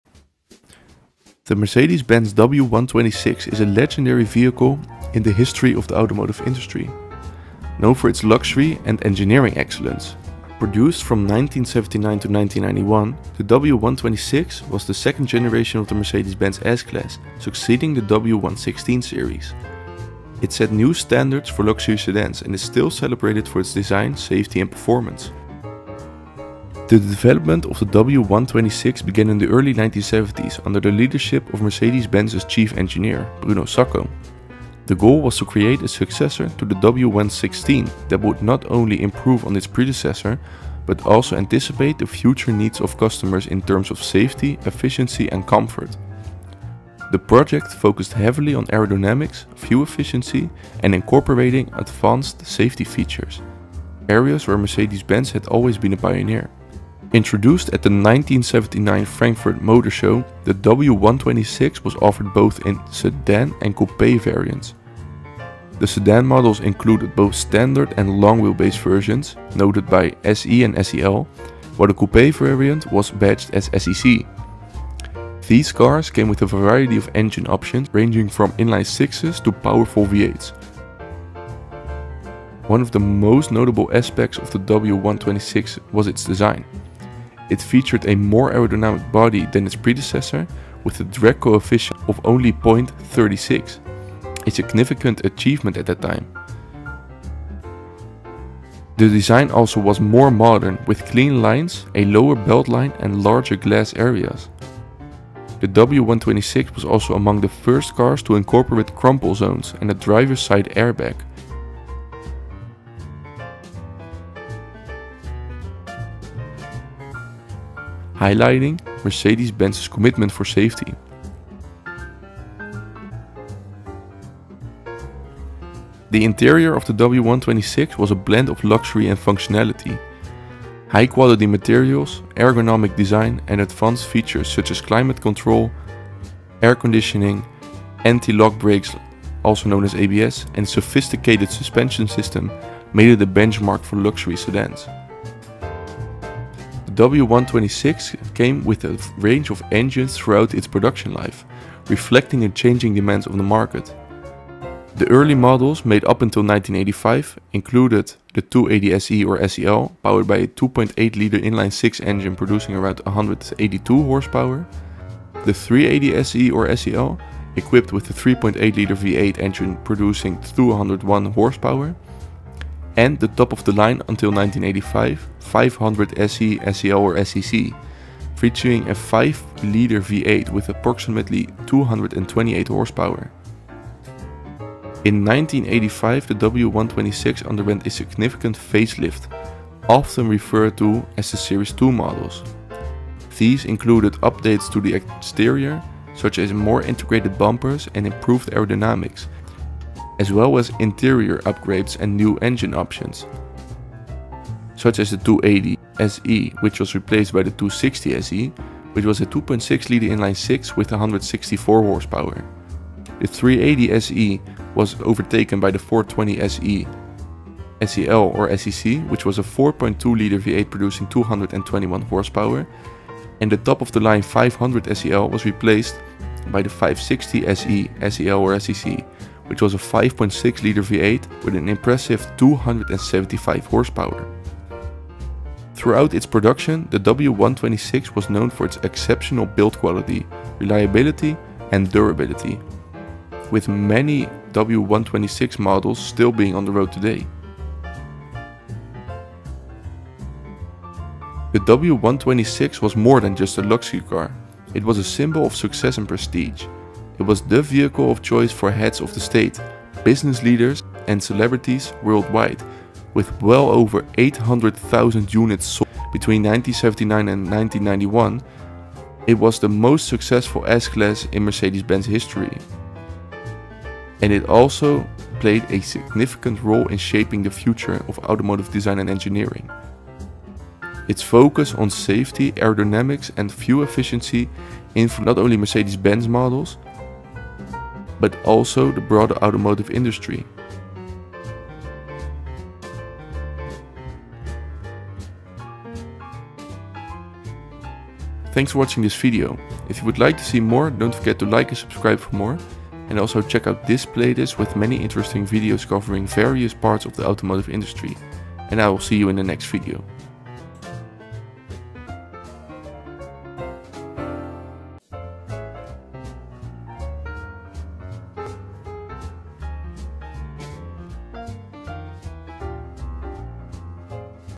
The Mercedes-Benz W126 S-Class Story - The Most Successful S-Class Ever Made
A car that was challenged with changing priorities by Mercedes-Benz and was revolutionary in so many different ways.

Let me know what car you want to see a video about next time and also watch this playlist down below for more interesting content!

0:00 Intro
0:26 Development
0:56 Introduction & Design
1:44 Safety Features
2:05 Interior
2:30 Engines
3:05 1985 Facelift
3:56 Build Quality & Summary
4:57 Outro

Back in 1979 the Mercedes-Benz W126 S-Class was introduced as the successor of the W116.
The car was in production till 1991 and 818.063 units were produced.
In 1985 the car got a facelift in which some engines were changed and the looks of the car changed a bit.
The top model; the 560 SEL was the most expensive model and had a 5.6-liter V8.
This model today, is the facelift of the W126 S-Class.
The wheelbase on the SEL models were 14 cm (around 6 inches) longer than the regular S-Class.
In this video the Carat Duchatelet Cullinan edition this car is fitted with, is reviewed.